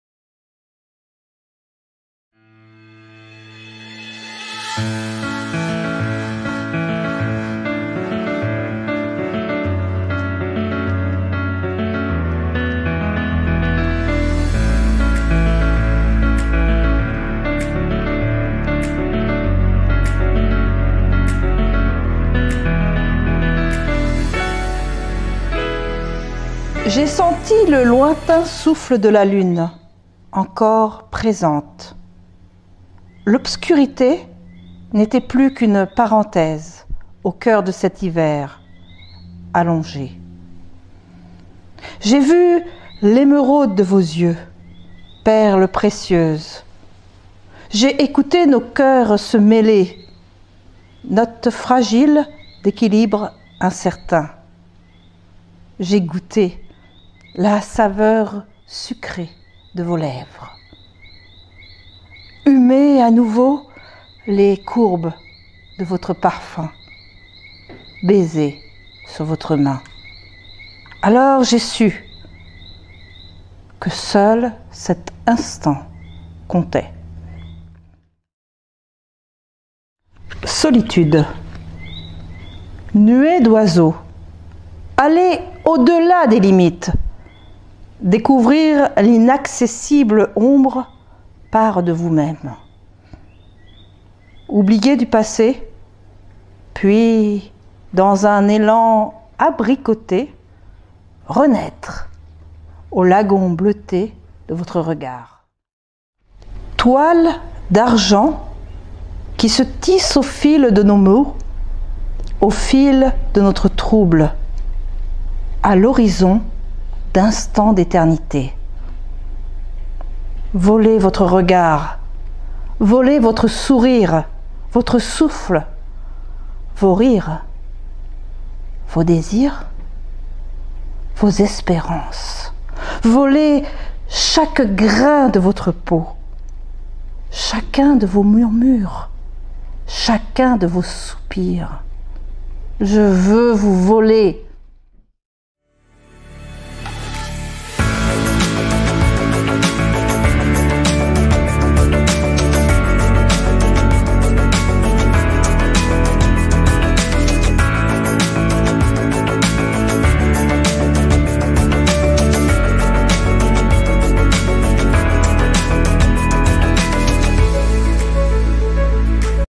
Marie galante au Printemps des Poetes 2021
Un jour, Un poème dévoile toutes les facettes du désir : jour 3 le désir de la Femme avec Lara et son recueil holographies